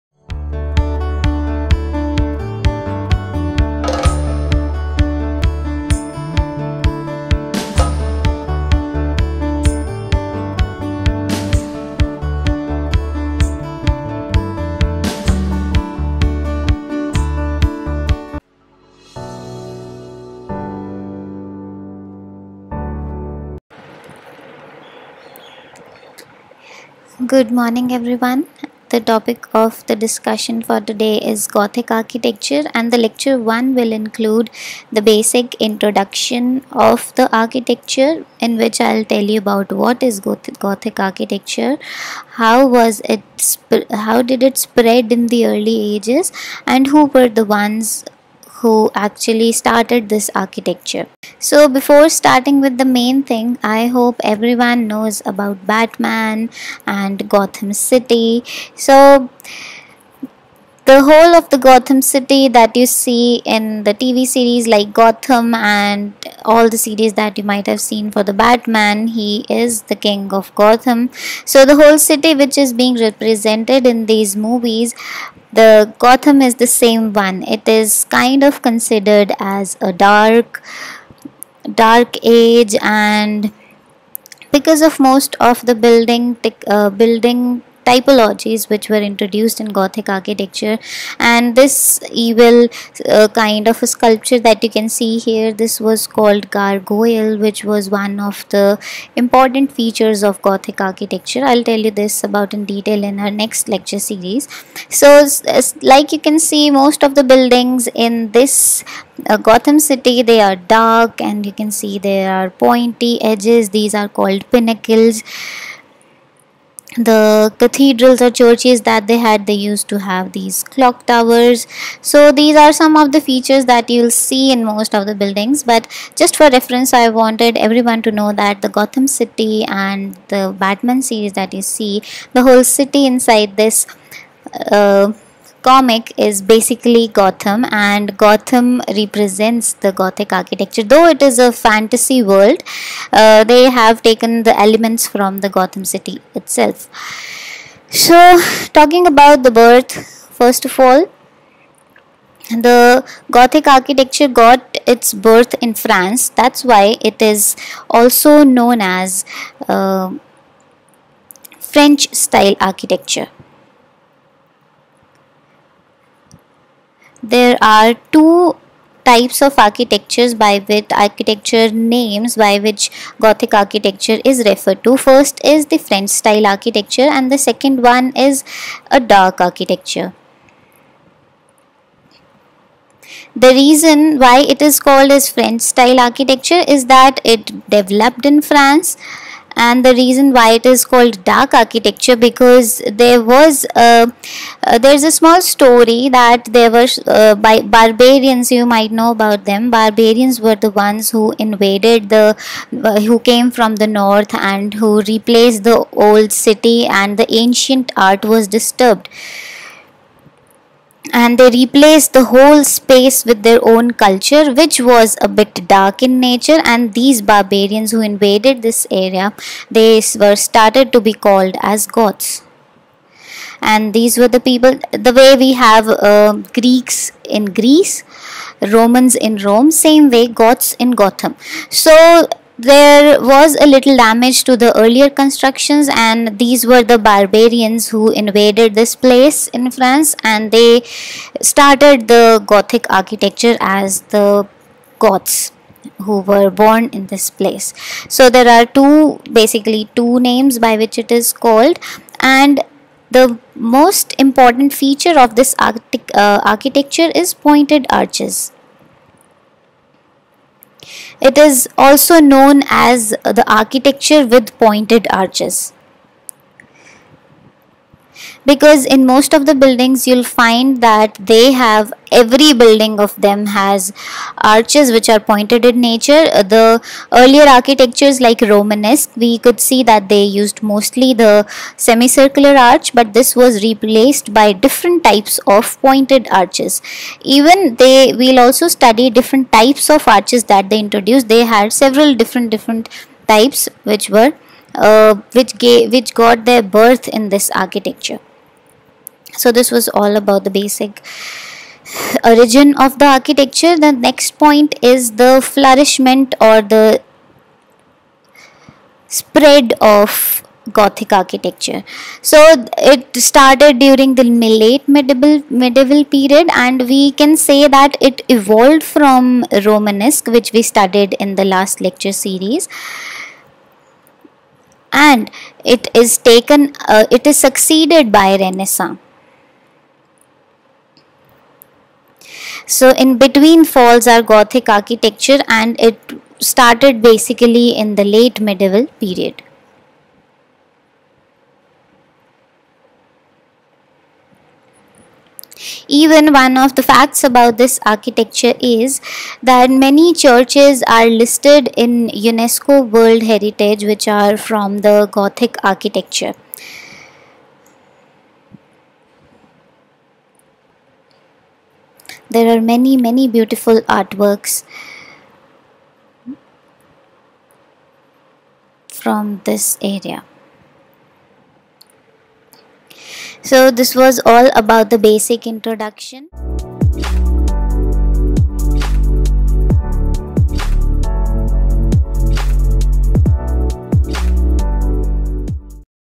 L1 Gothic Architecture I Introduction: What, Why, How ? I Architecture Lecture Series
Gothic Architecture:

Birth: Gothic architecture was at first called "the French Style” as it flourished more in France.

Today the word "Gothic" to describes architecture with pointed arches.
Characterisation: It is characterised by use of the pointed arch. Other features common to Gothic architecture are the rib vault, buttresses, including flying buttresses, large windows which are often grouped, ornate façades.